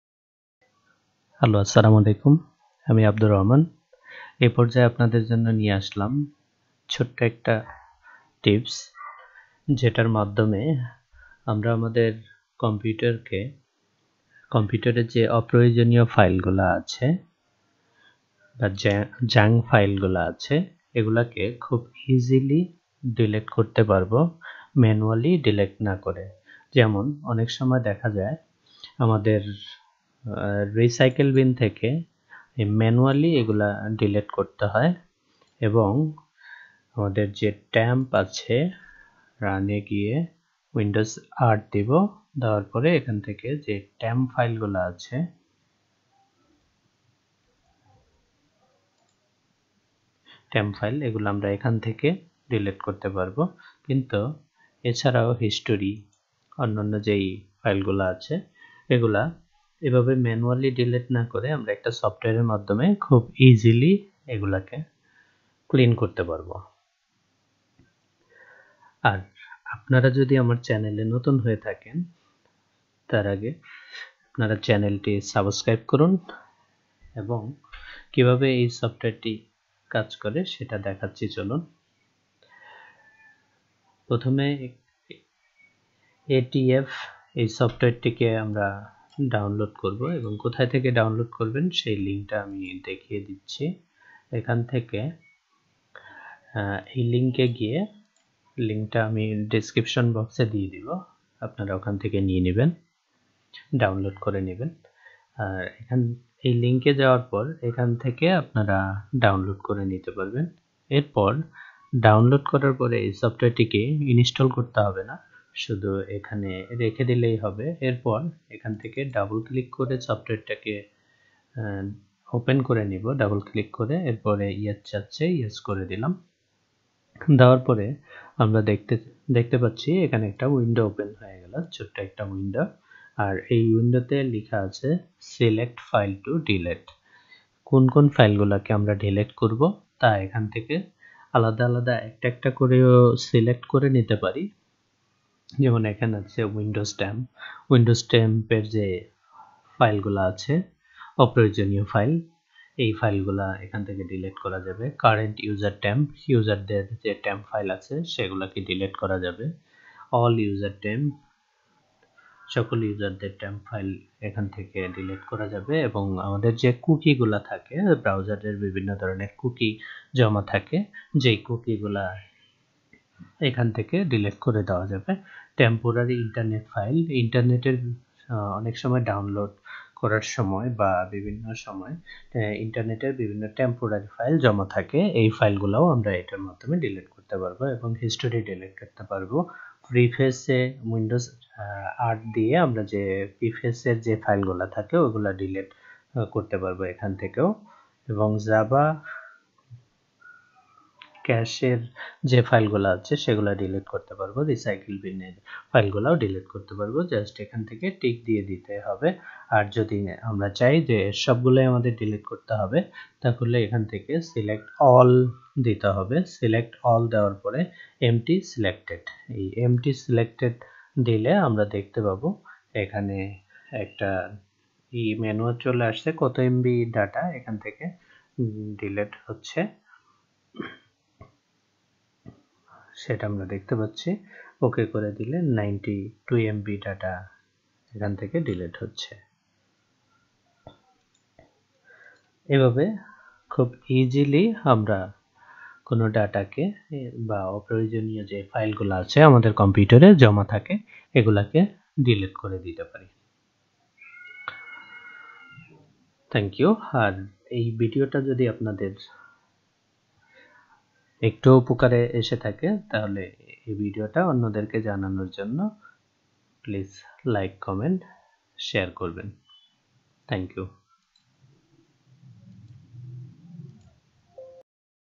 How can we Delete All Unwanted File or Junk Files From our Computer !!!
Easy Way to Remove Computer all Junk Files or Unwanted file:
Windows Temp
Current User Temp
All Users Temp
Cookies
Temporary Internet Files
History File
Prefetch File
Java Cache
Recycle Bin File